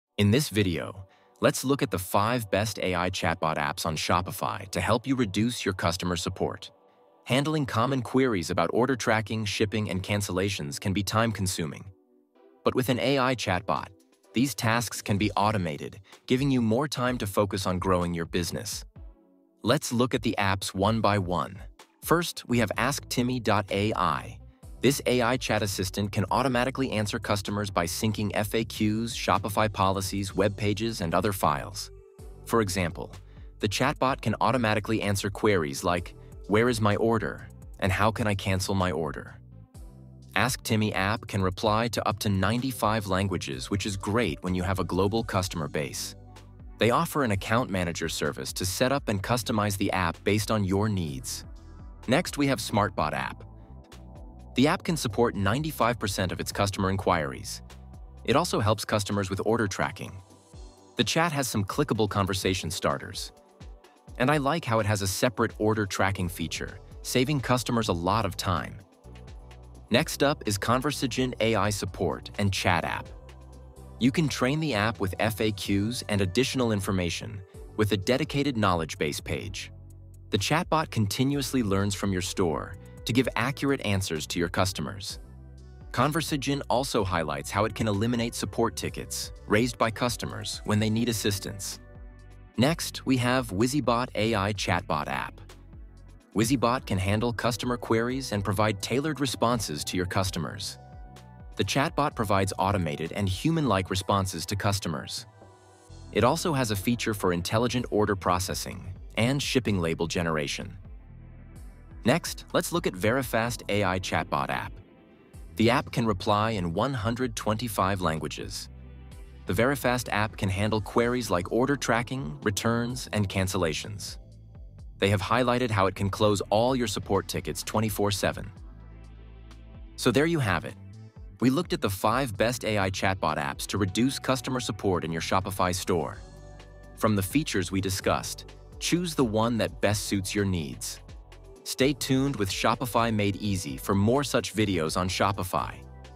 5 Best AI Chatbots for Shopify to Reduce Customer Support | Feature Comparisons | 2026
In this video, you will learn about the features of the 5 Best Shopify Chatbot apps to help you reduce customer support on your store.

Key moments
0:00 - Intro
0:22  - AskTimmy.ai
0:52 - Smartbot
1:09 - Conversagent
1:32 - Wizybot
1:51 - Verifast